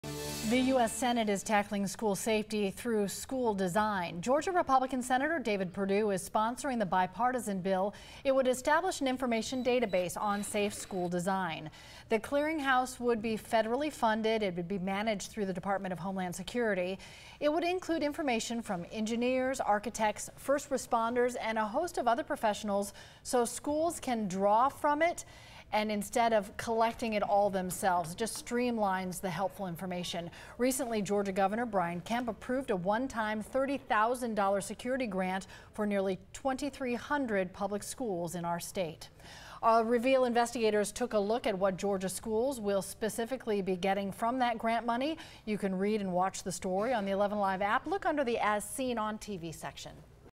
Perdue sponsors bill that would create database on safe school design
Here is more on the bill.